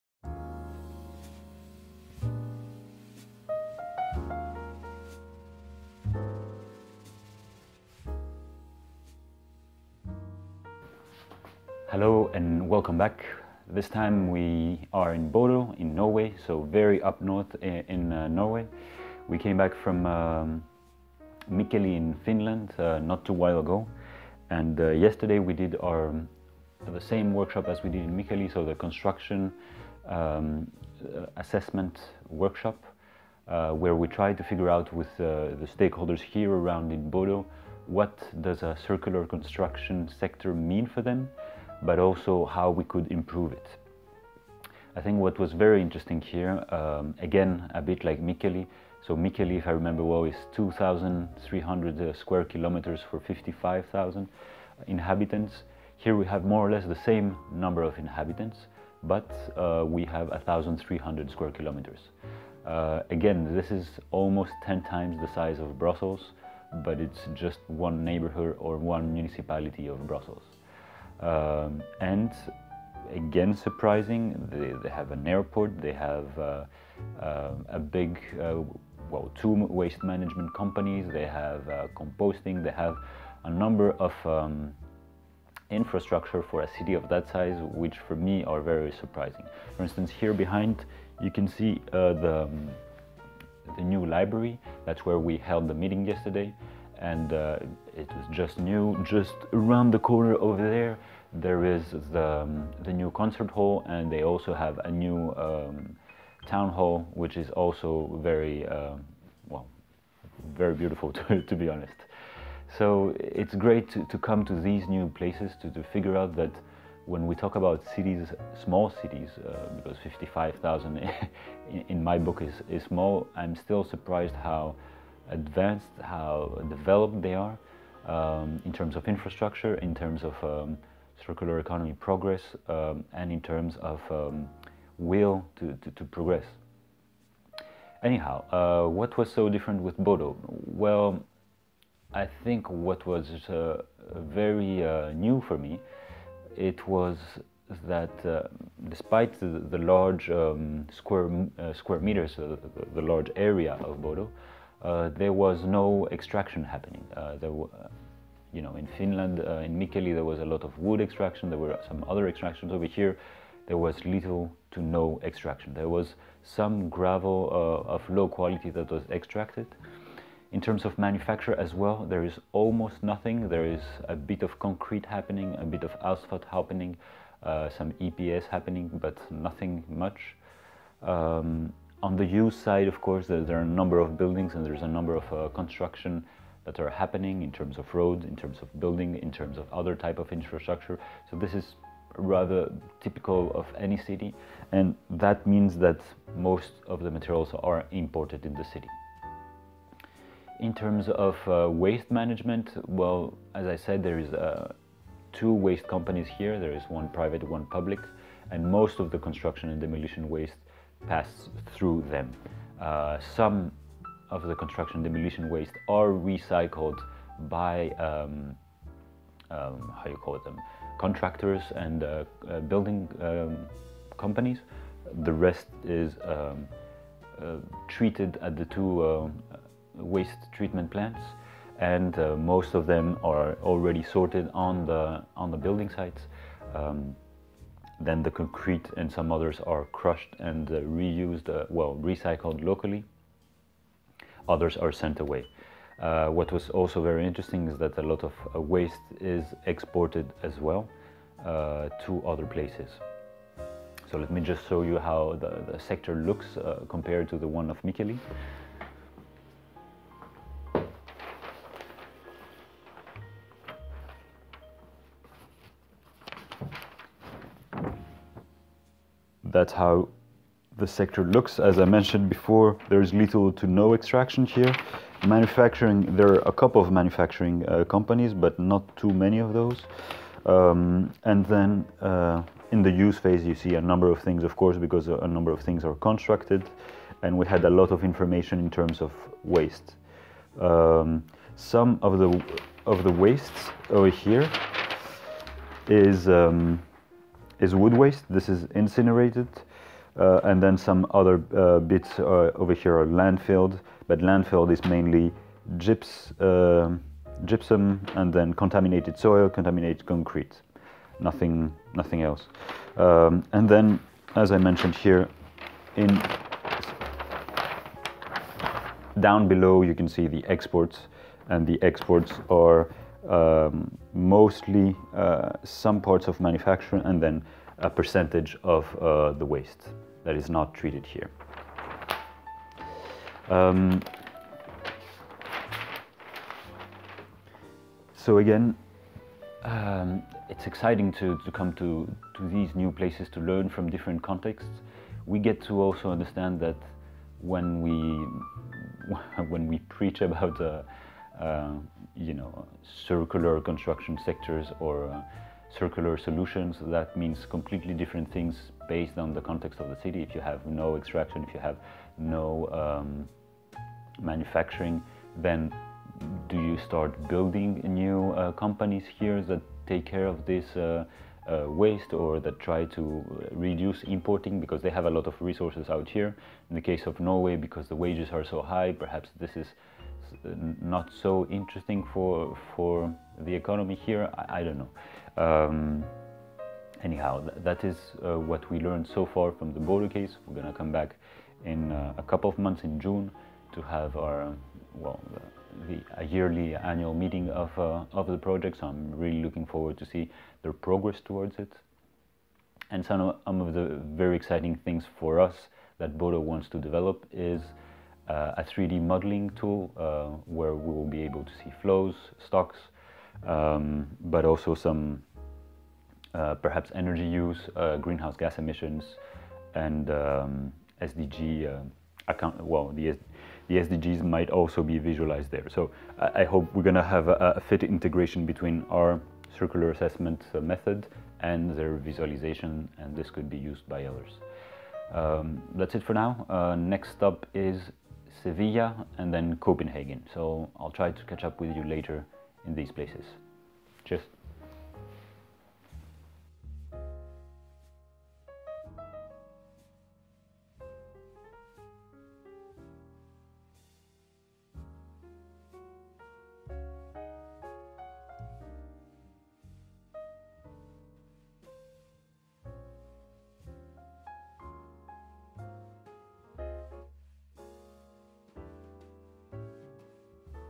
Exploring circularity in the construction sector in Bodø, Norway
Within the framework of the H2020 project CityLoops, we explored the circularity of the construction sector in Bodø, Norway. In this video we show how we look for different pieces of information to construct a puzzle of flows/actors/infrastructure in order to best assess the circularity of these sectors.